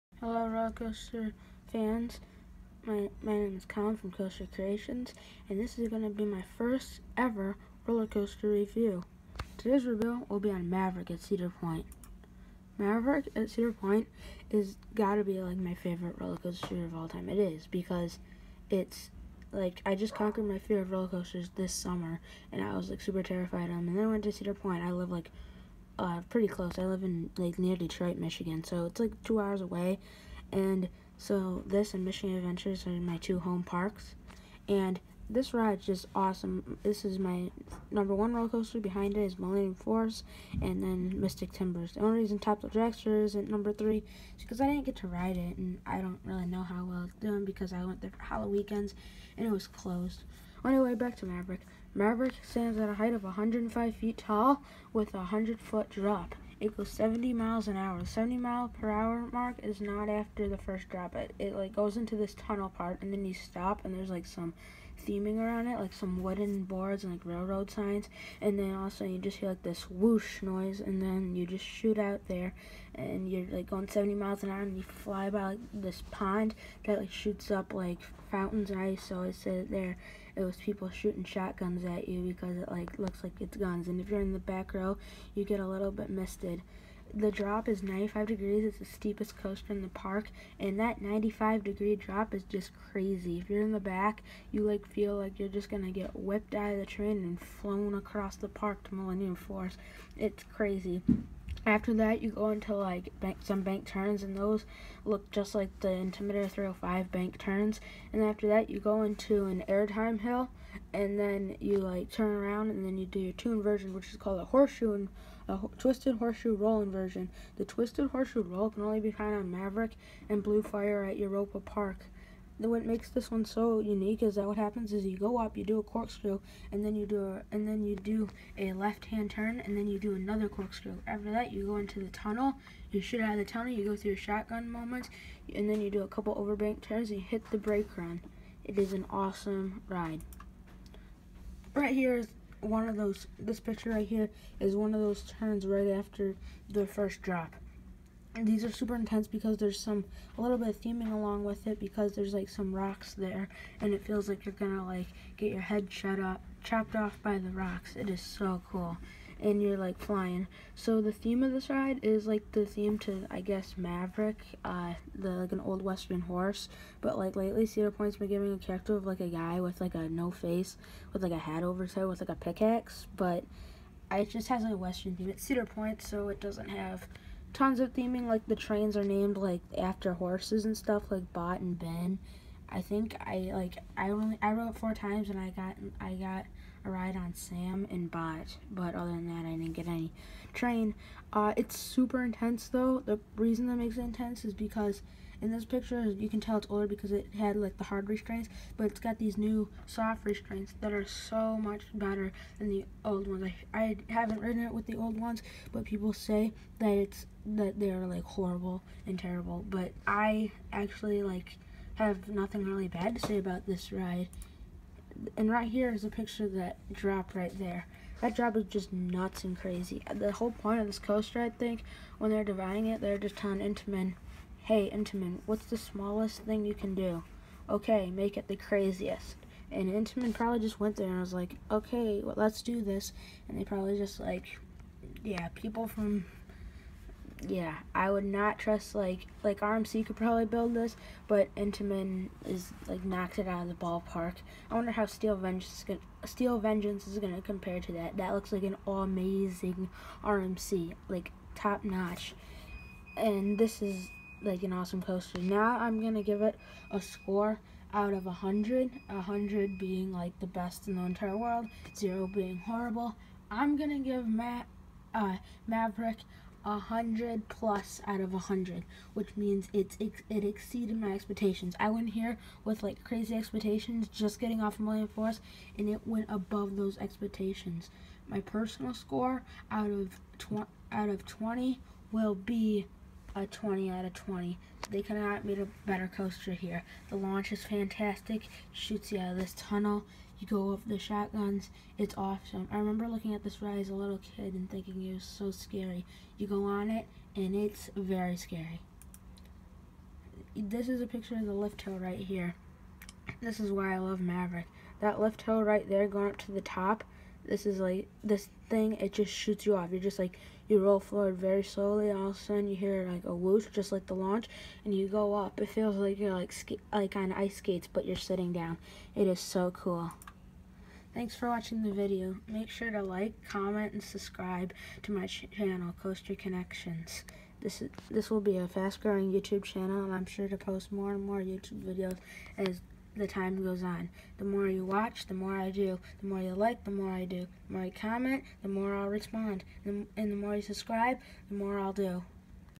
Maverick review cedar point
Is a review and Maverick at Cedar point in Ohio USA. This ride is super intense it is easily my favorite roller coaster watch this video to find out why!